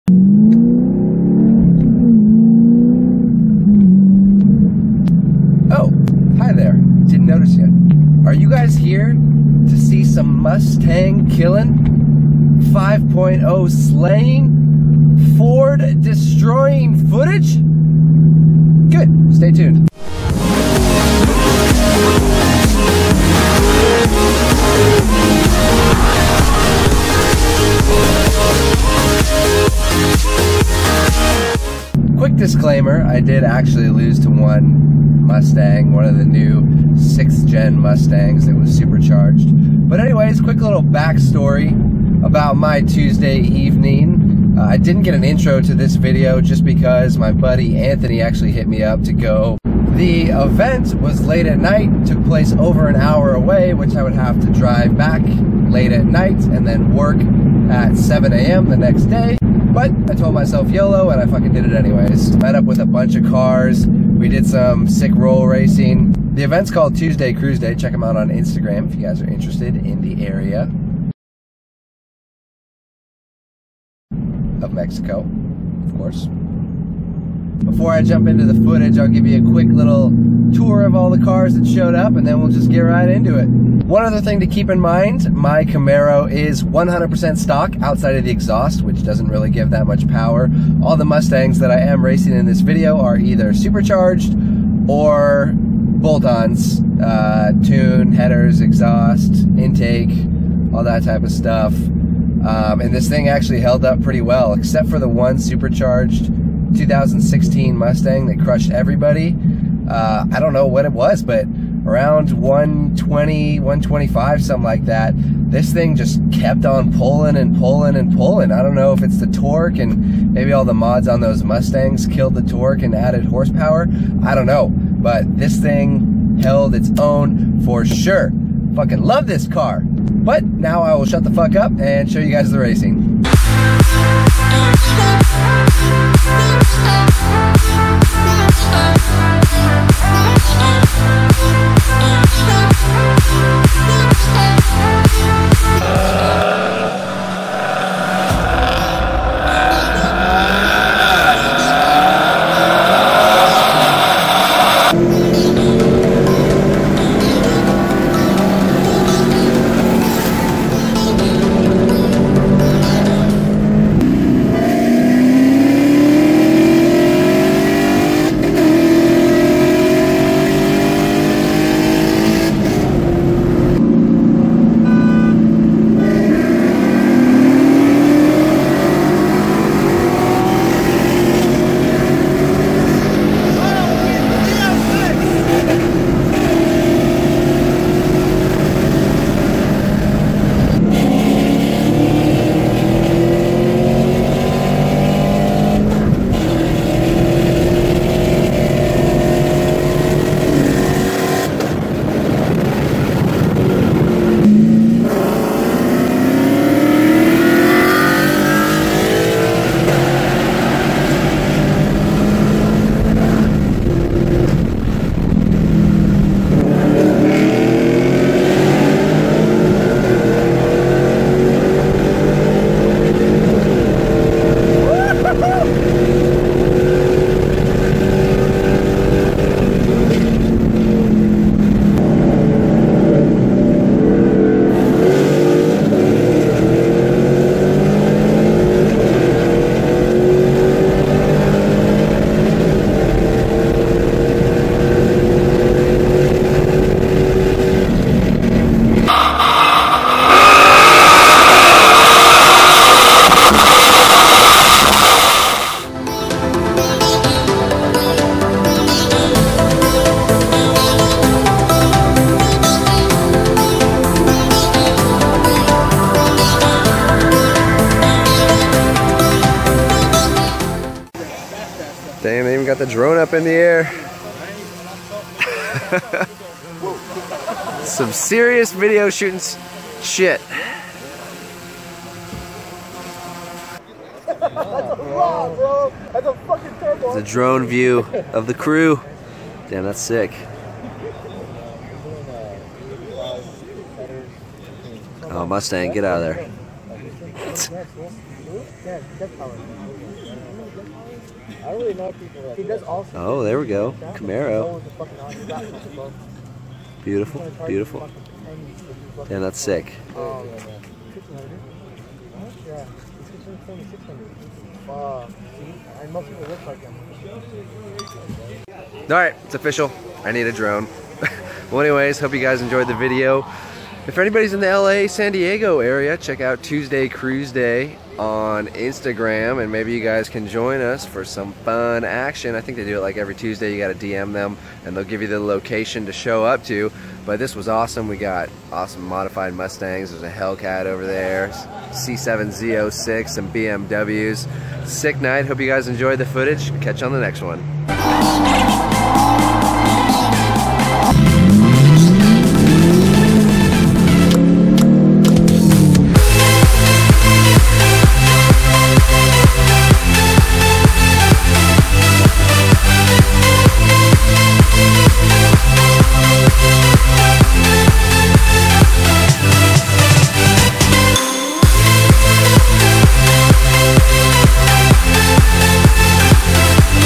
STOCK Camaro SLAYS Boosted Mustang
Did some roll racing down "in Mexico" and beat on a couple modded Mustangs.

Race 1 vs Turbo 5th gen Mustang
Race 2 vs Supercharged 6th gen Mustang
Race 3 Chase Cam with 4 Mustangs
Race 4 vs Full bolt on 5th gen Mustang

I noticed that my stock Camaro seemed to pull on all of them above 120mph except for the supercharged 6th gen. I'm guessing that the modded stangs lost torque with their mods and can't keep up in the higher speeds vs the Camaro. Enjoy!